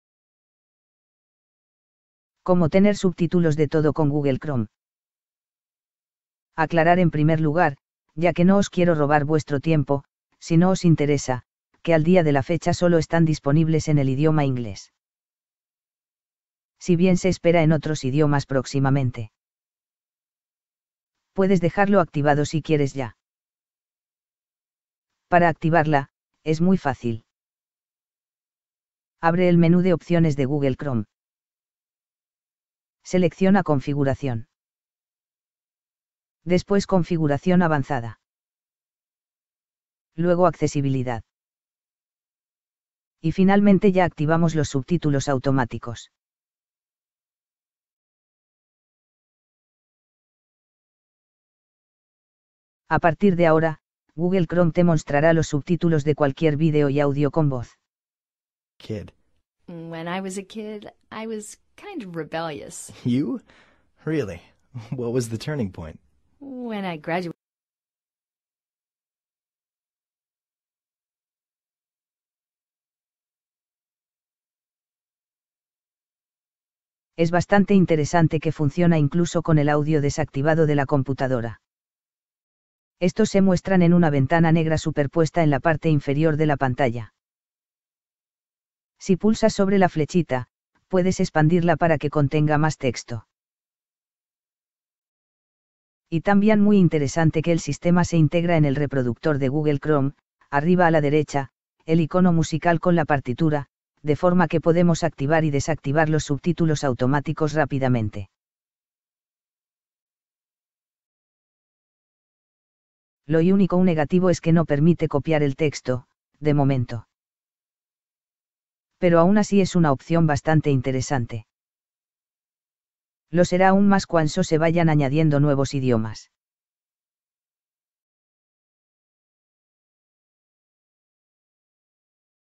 SUBTÍTULOS automáticos en Chrome de audio y vídeo.
Los subtítulos automáticos en tiempo real de Google Chrome ya están disponibles: así funcionan y se activan.

Aclarar en primer lugar, ya que no os quiero robar vuestro tiempo, si no os interesa, que al día de la fecha solo están disponibles en el idioma inglés. SI bien se espera en otros idiomas próximamente. Puedes dejarlo activado si quieres ya!

Para activarla, es muy fácil.

Abre el menú de opciones de Google Chrome

Selecciona "Configuración".

Después "Configuración avanzada".

Luego "Accesibilidad".

y finalmente ya Activamos los subtítulos automáticos.

 A partir de ahora, Google Chrome te mostrará los subtítulos de cualquier vídeo y audio con voz.

Estos se muestran en una ventana negra superpuesta en la parte inferior de la pantalla. Si pulsas sobre la flechita, puedes expandirla para que contenga más texto.

Es bastante interesante que funciona incluso con el audio desactivado de la computadora.

Y Tambien muy interesante que el sistema se integra en el reproductor de Google Chrome (arriba a la derecha, el icono musical con la partitura), de forma que podemos activar y desactivar los subtítulos automáticos rápidamente.

Lo Unico negativo es que no permite copiar el texto, de momento. Pero aun así es una opción bastante interesante. Lo será aún más cuando se vayan añadiendo nuevos idiomas.

Cómo activar los subtítulos automáticos de Google Chrome para que aparezcan en tiempo real en Instagram, YouTube, Netflix y más.
Google activa los subtítulos automáticos para audio y vídeo en el navegador así que es posible transcripciones de audio a texto. Utilizar Subtítulos automáticos ¿Tutorial de cómo se activan los subtítulos automáticos? un magnífico tip